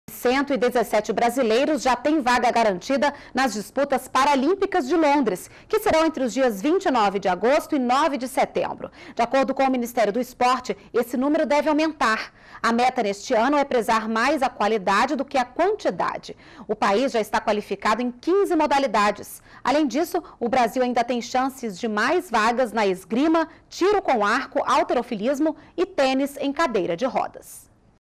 Brasil já tem 117 vagas garantidas nas disputas Paralímpicas de Londres em 2012
NBR Notícias - 21.02.12: Brasil têm vaga garantida  para 117 atletas nas disputas Paraolímpicas de Londres, que serão entre os dias 29 de agosto e 9 de setembro. De acordo com o Ministério do Esporte, esse número deve aumentar. A meta neste ano é prezar mais a qualidade do que a quantidade. O país já está qualificado em 15 modalidades. Além disso, o Brasil ainda tem chances de mais vagas na esgrima, tiro com arco, halterofilismo e tênis em cadeira de rodas.